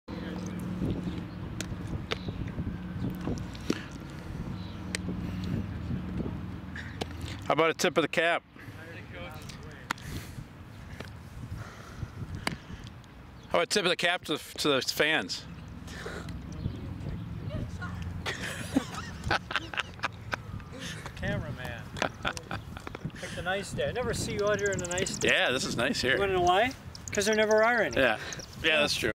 2014/06/03 16.25 Mitch Pregame, Mandi Shocked
Around 0:17 you can hear Mandi yell "Mitchell" when she first sees his new hair cut.